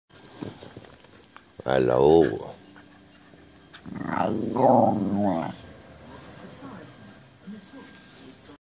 Dora hello.
Dora is a wire-hair terrier that will do and say just about anything for a cup of coffee. .